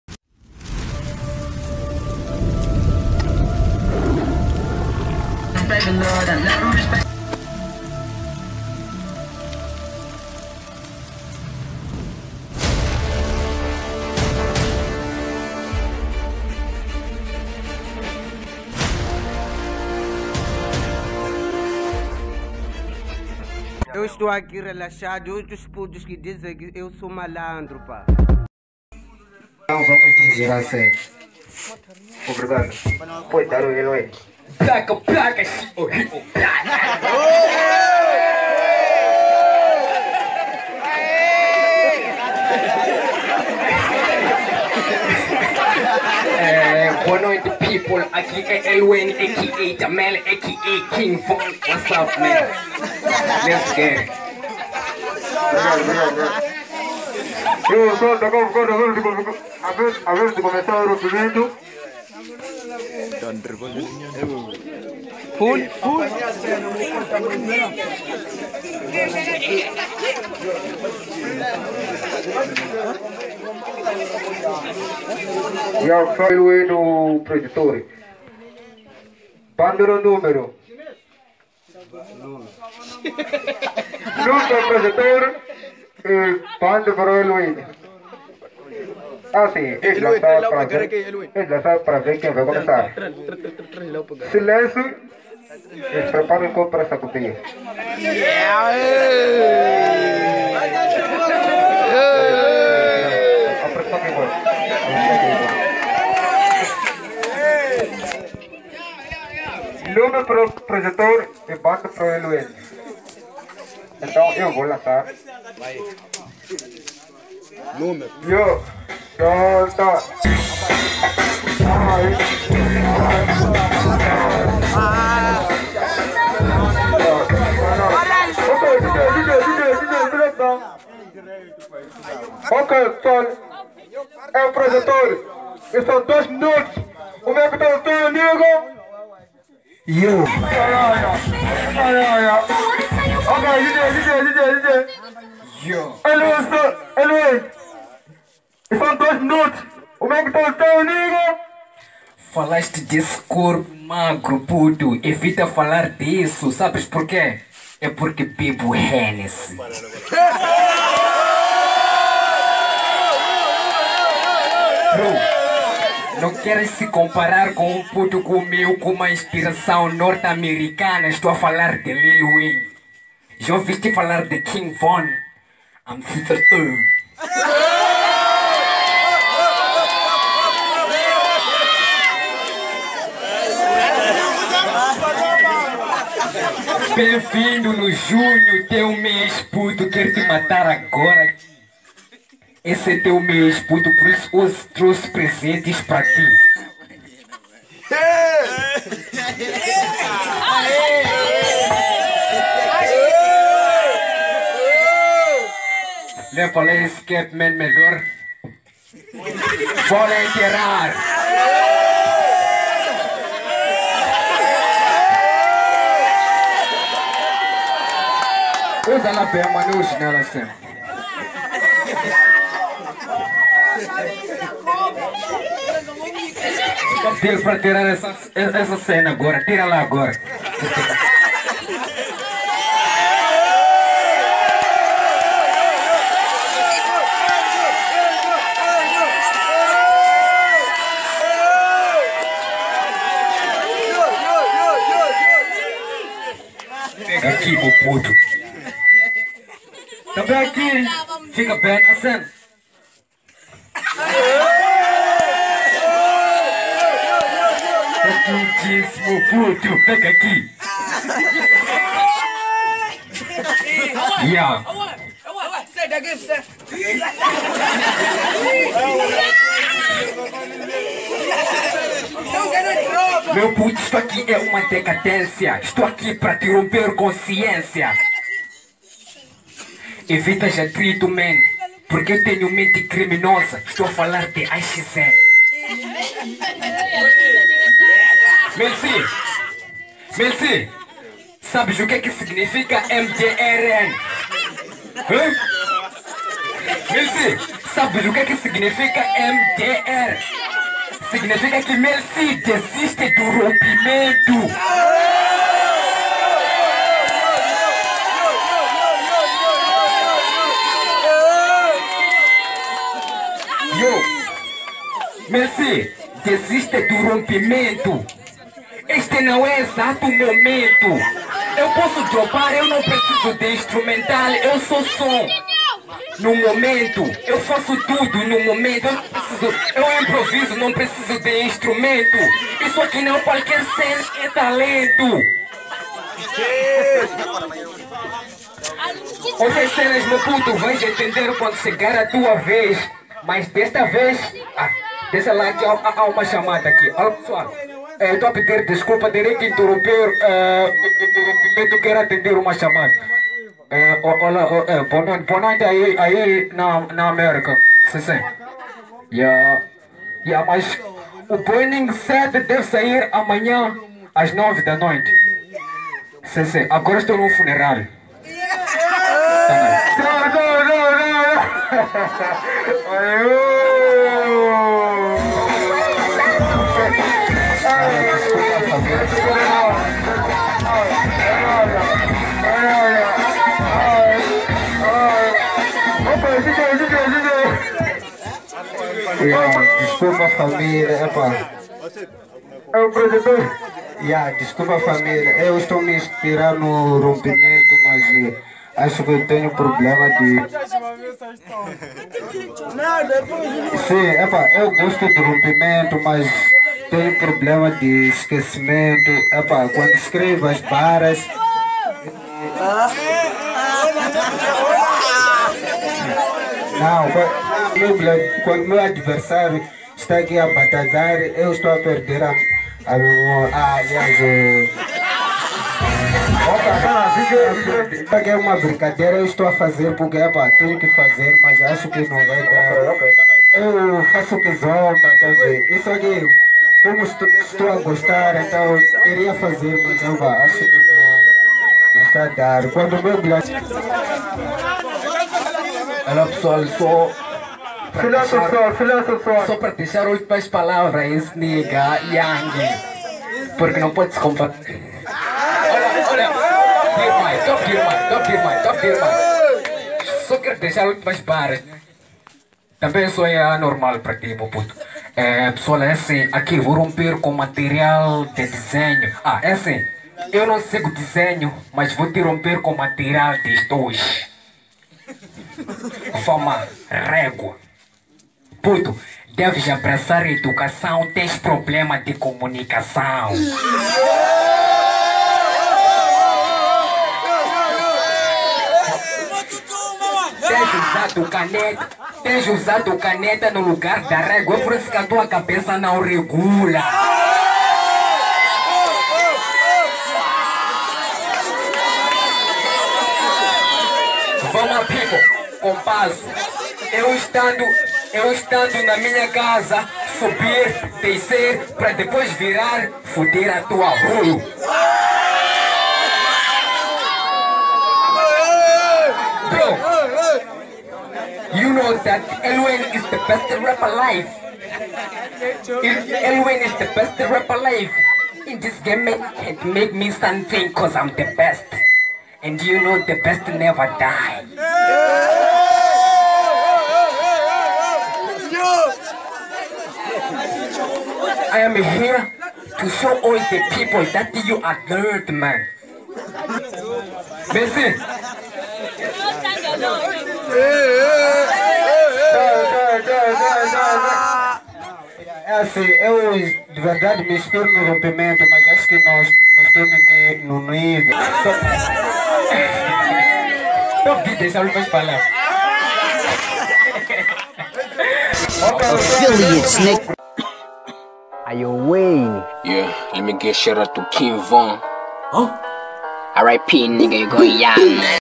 Best Moments Of Rap Battle | EL Wayne VS Projector Melsy| RRPL| SLRM| Manjacaze| Rap Moz
Hello Everyone
Devolta para Apresentar Mais um trabalho feito com amor!
[EL Wayne VS MËLSY]
Batalhada no dia 10 De Junho 2021 Em Zonduene, Vila de Manjacaze[Gaza]